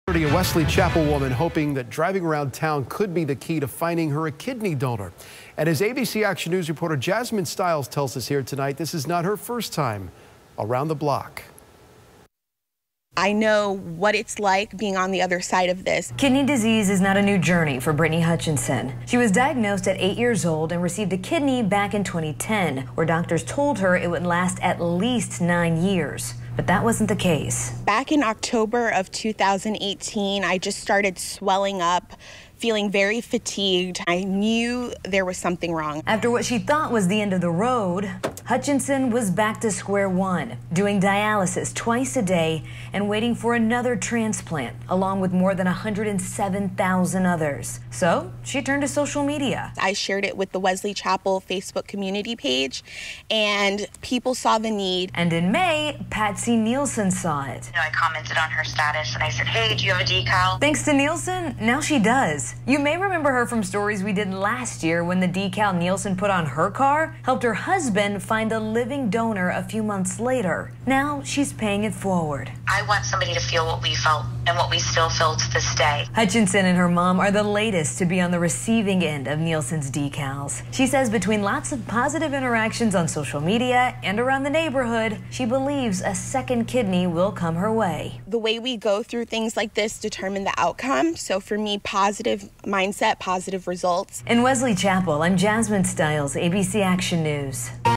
Woman uses car decal to find a donor for her second kidney transplant
A Wesley Chapel woman hopes driving around town could be the key to finding her a kidney donor.